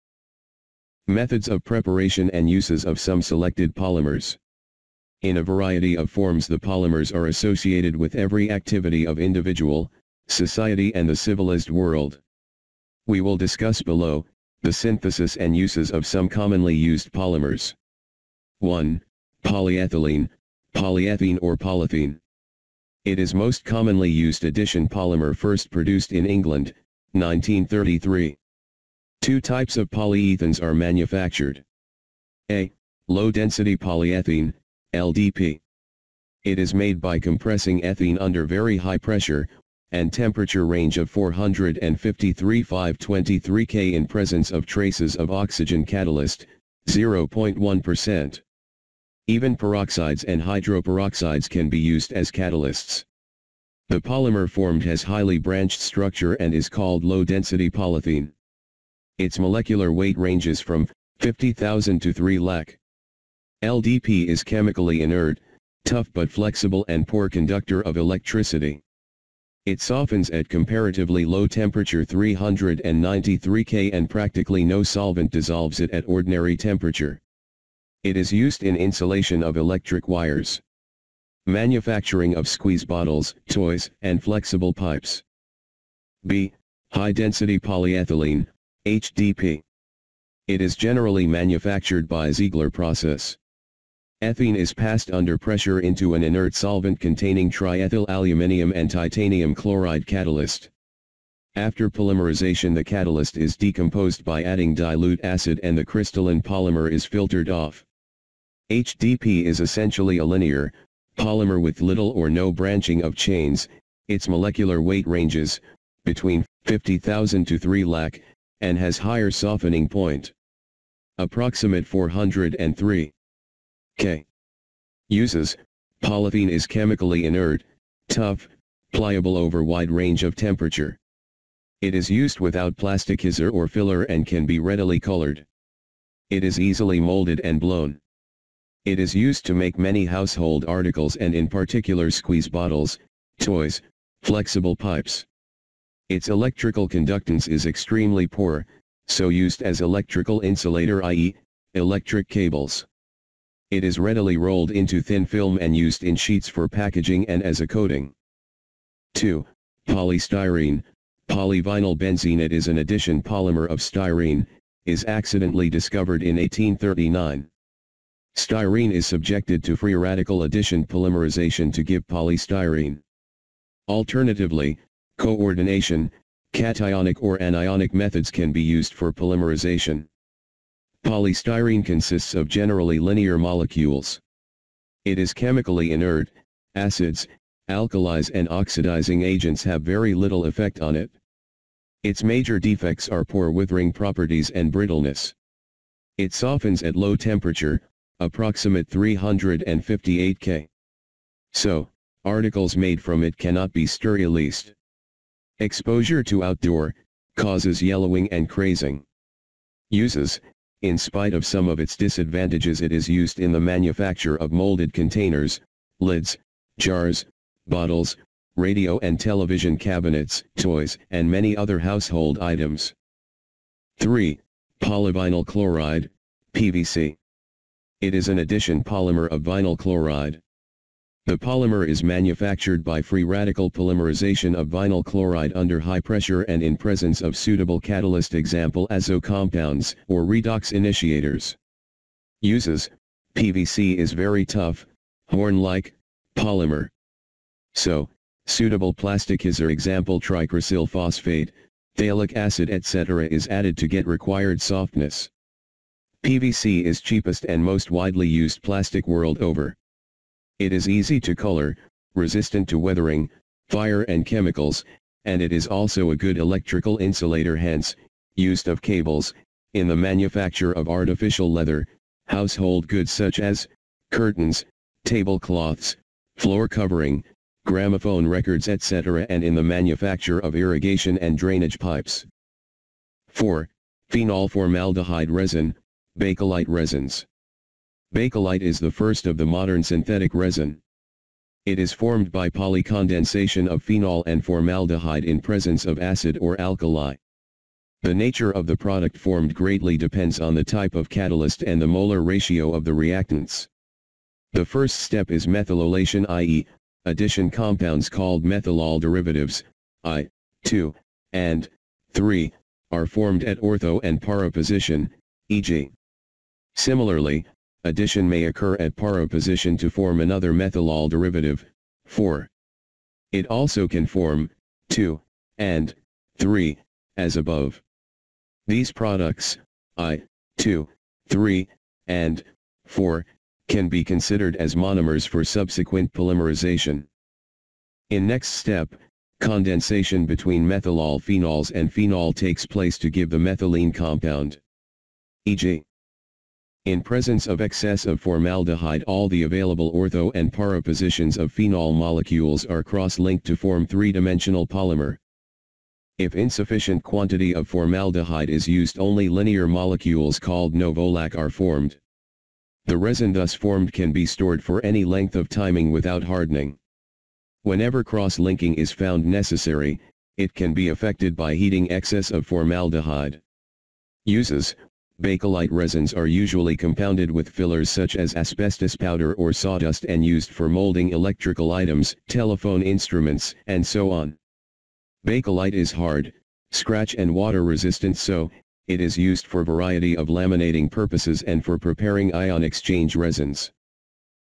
Polythene (LDP,HDP), Polystyrene, Polyvinyl chloride (PVC), Bakelite resins: Preparation and Uses
1) Polyethylene (Poly-ethene or polythene):
(a) Low density polyethene (LDP): Free radical addition polymerization.
(b) High density polyethylene (HDP): Ziegler-Natta Polymerization.
2) Polystyrene (poly vinyl benzene): Free radical addition polymerization.
3) Polyvinyl chloride (PVC): Free radical addition polymerization.
4) Phenol-formaldehyde resin(Bakelite resins): Condensation polymerization.